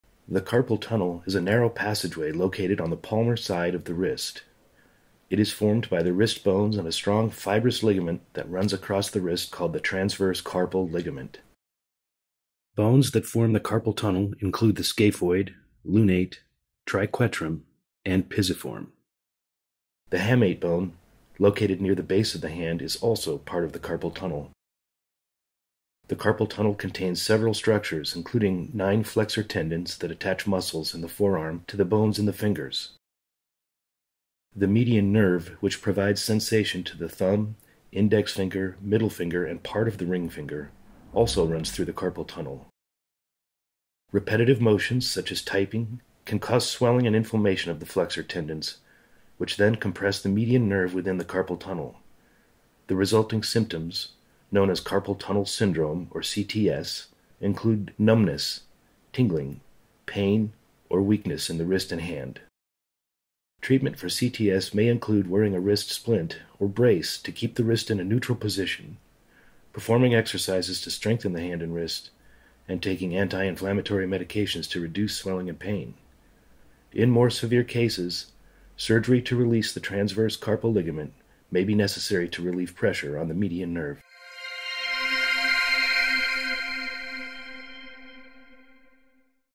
Dissection in 3D: Carpal Tunnel
Learn about the structures packed in this small area of the body, and what might go wrong.